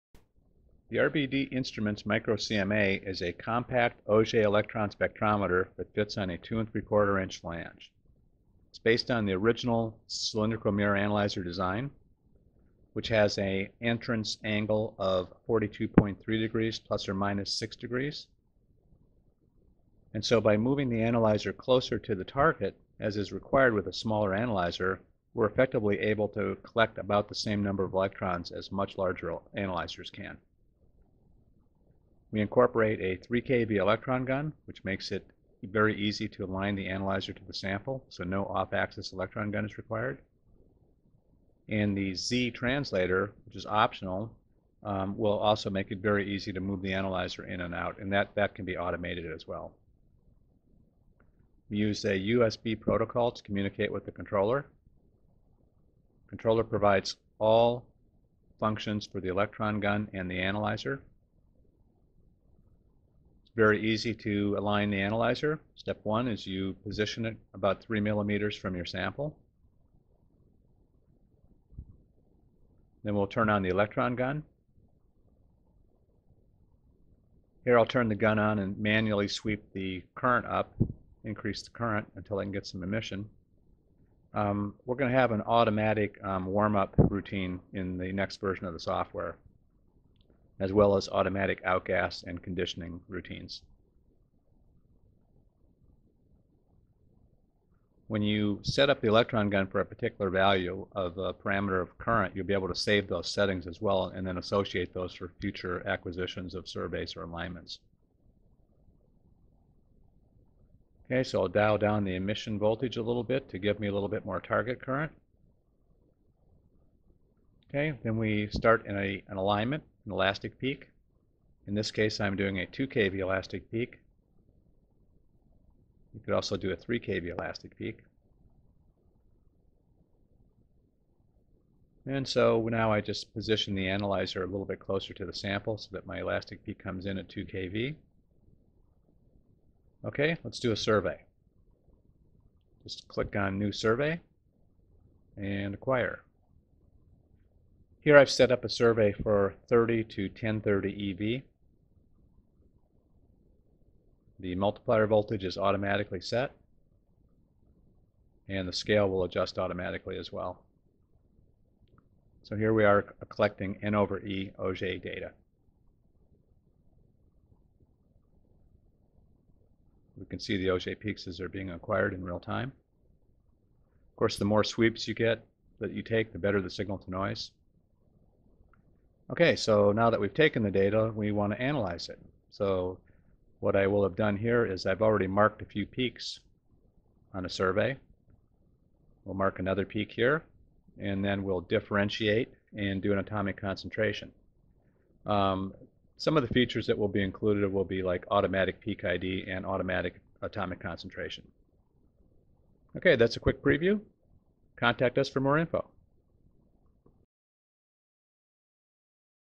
microCMA mini demo
Brief description of the RBD Instrument's microCMA compact cylindrical mirror analyzer used for Auger Electron Spectroscopy. The microCMA fits on a 2.75" CF flange and incorporates a 3kV coaxial electron gun for easy CMA to sample alignment. The software interfaces with the controller via a USB interface.